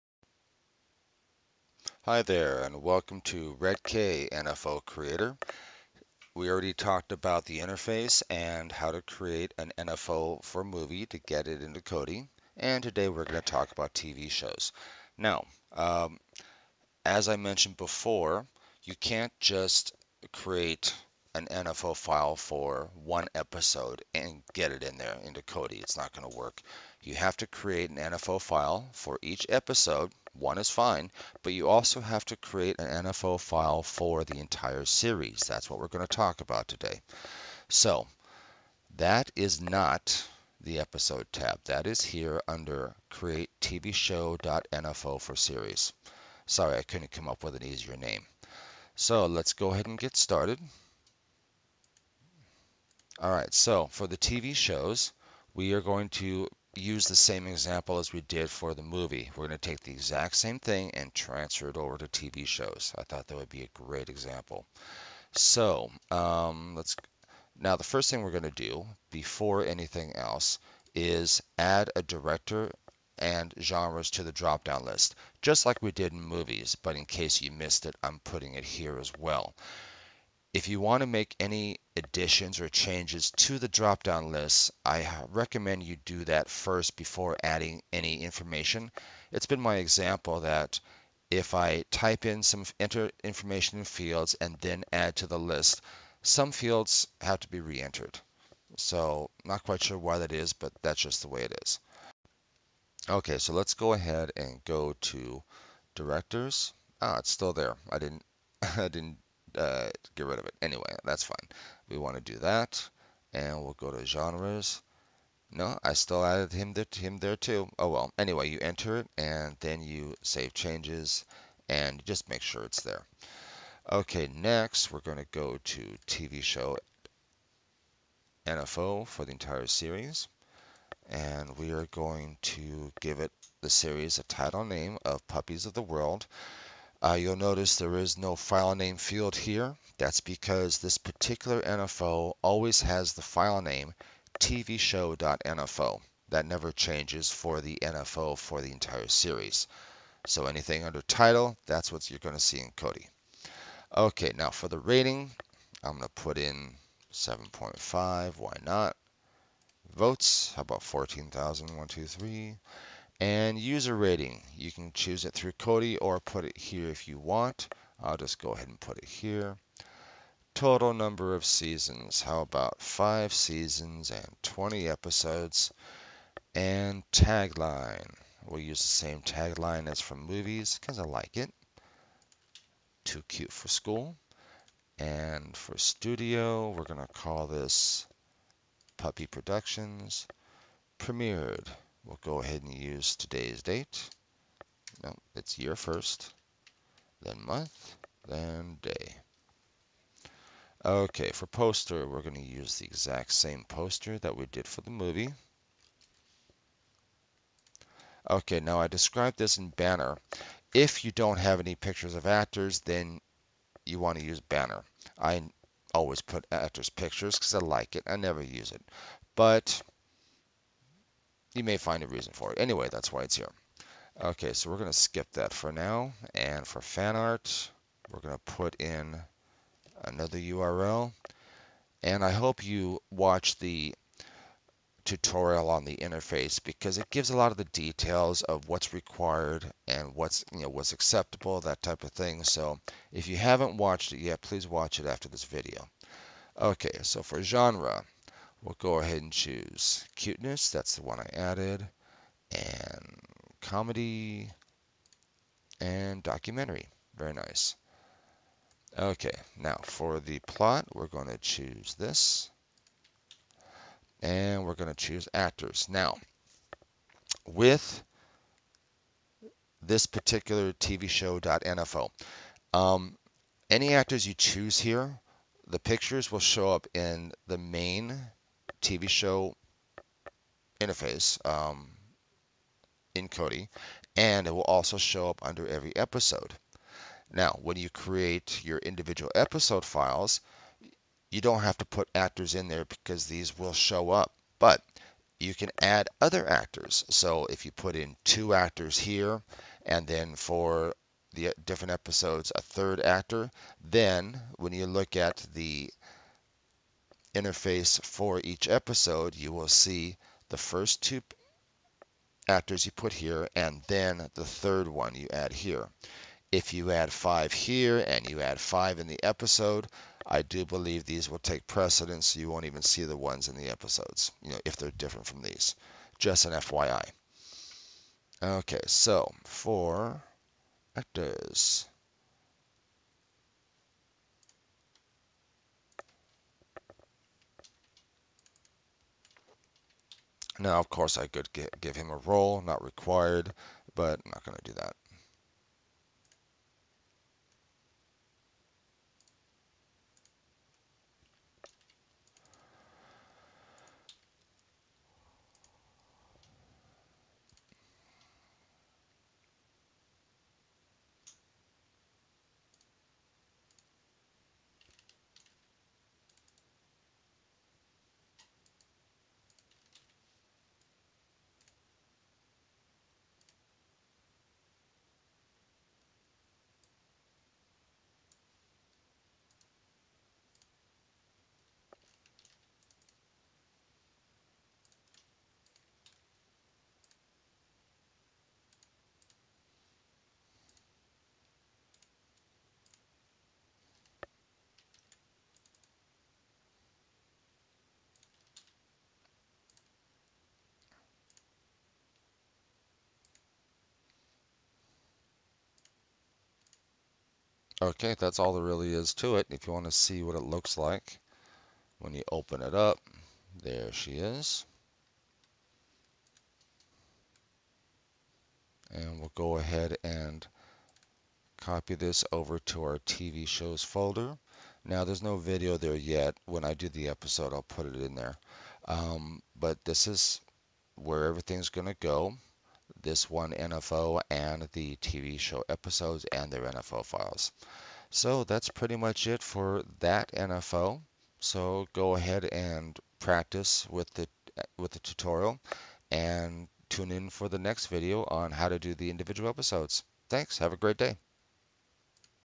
4 Red-K NFO Creator - Tutorial Create NFO files for TV Show Series
Learn how to create NFO files for a TV Series with Red-K NFO Creator